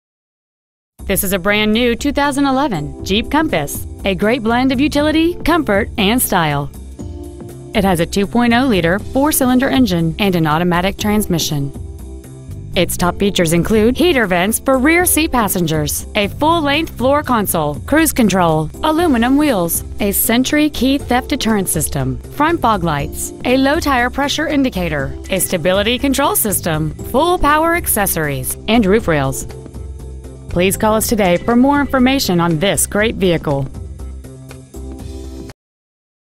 2011 Jeep Compass Austin TX 78745
We are proud to present this 2011 Jeep Compass .

 Year : 2011
 Make : Jeep
 Model : Compass
 Engine : 2.0L DOHC 16V I4 dual-VVT engine
 Trans . : Automatic
 Exterior : Red

 Stock : 176693

 5210 South I H 35